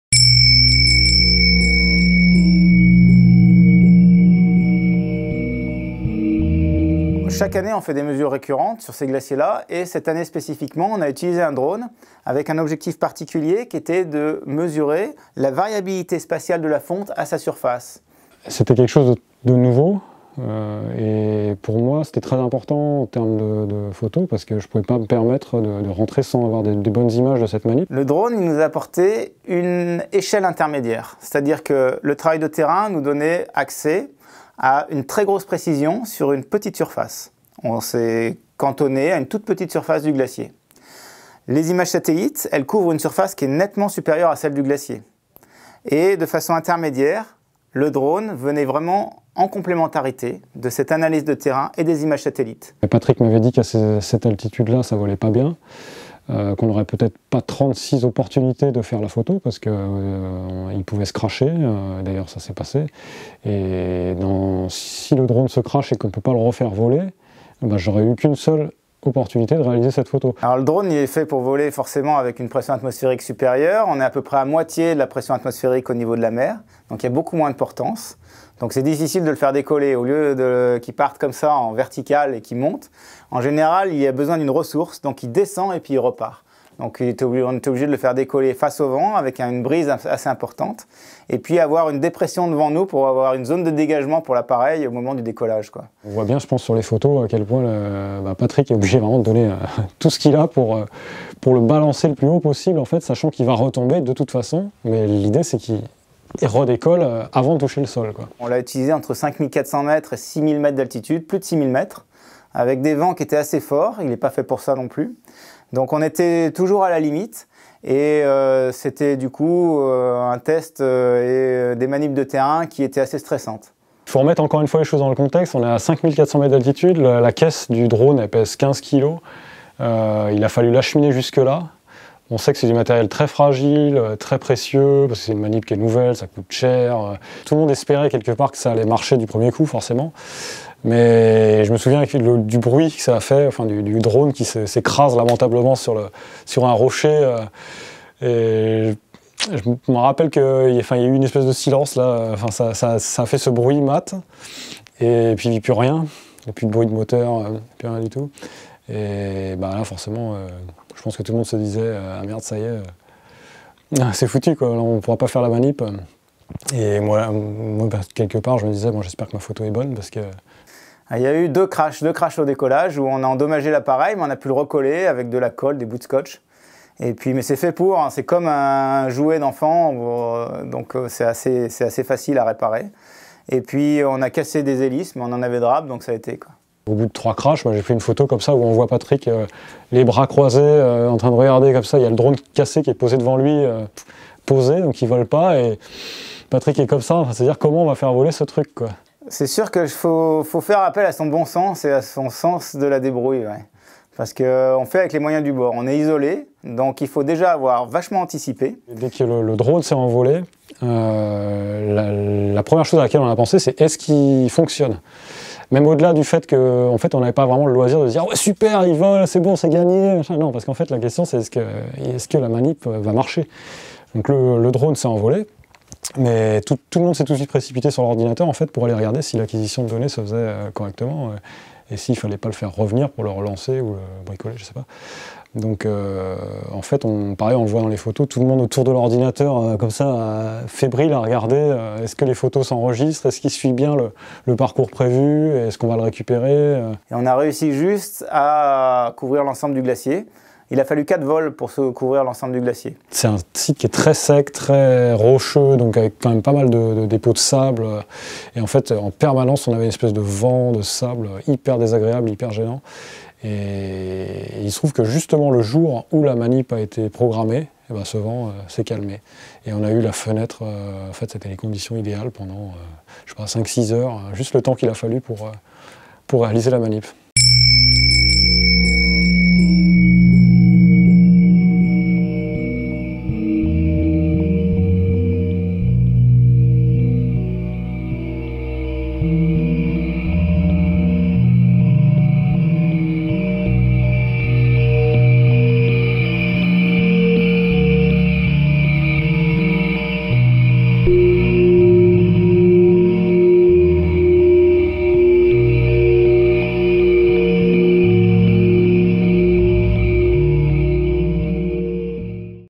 Vu du ciel
Patrick Wagnon, glaciologue au sein de l’UMR 252 (Institut de géosciences de l’environnement), et Thibaut Vergoz, photographe, évoquent les prouesses nécessaires pour faire voler un drone à plus de 5 000 mètres d’altitude et photographier cette expérience inédite. L’objectif de la manœuvre : survoler et cartographier, en complément des images satellites et des mesures de terrain, le site du Changri Nup, un des glaciers noirs d’Asie, étudié dans le cadre du programme de recherche interdisciplinaire PRESHINE (IRD/CNRS).

Réalisation : Hubert Bataille / Tatiana Vincent